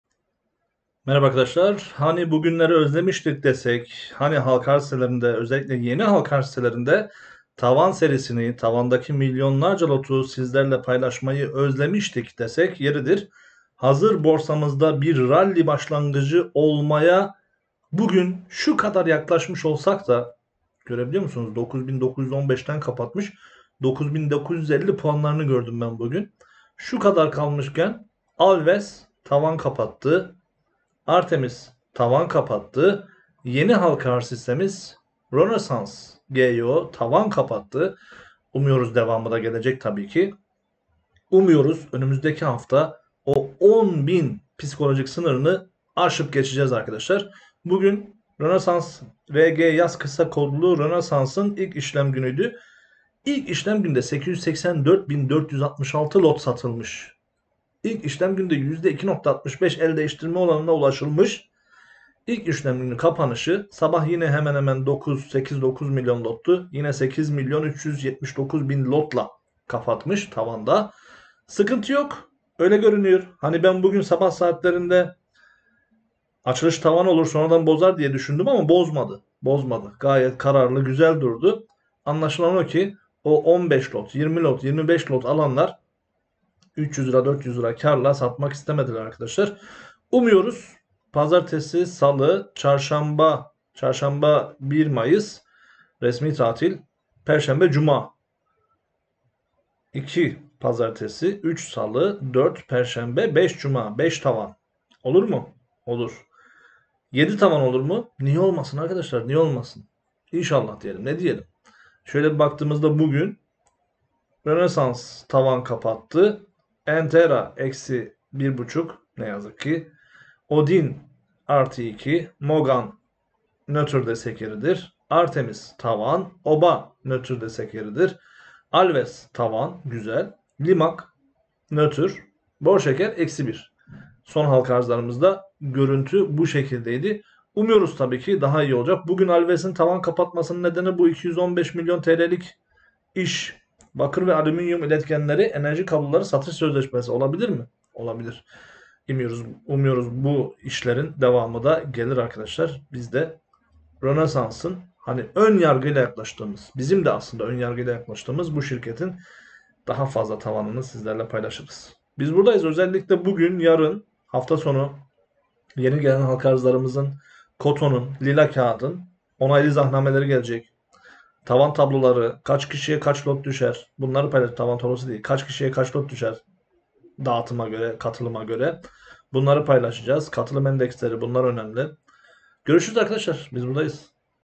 Halka Arz Hisselerinde Hafta Kapanışı alves entra rgyas Tavan Serilerine Start Verdik Bakalım
Halka arz hisselerinde nihayet özlediğimiz rakamlar gelmeye başladı. Tarihi zirveleri zorlayan borsa yanında halka arz hisselerinde tavan serileri de tekrar başladı.